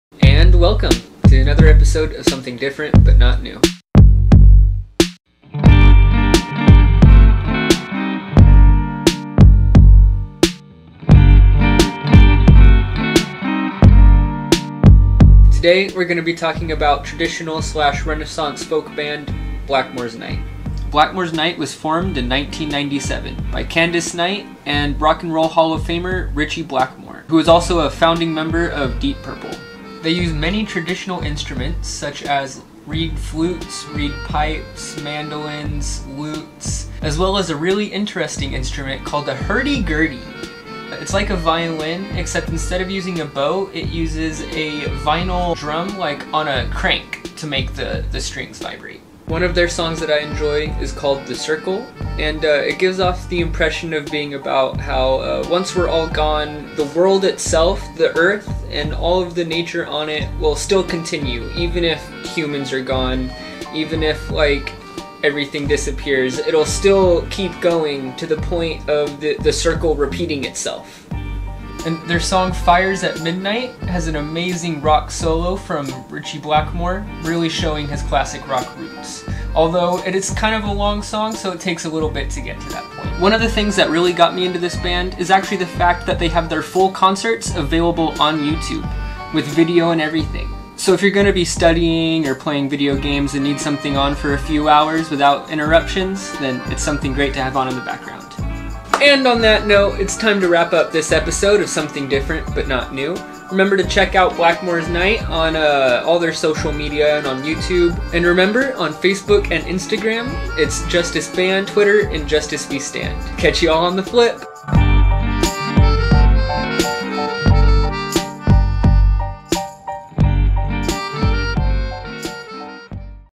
The Circle Repeats
In this episode of, "Something Different but Not New", Cody talks about the band, Blackmore's Night.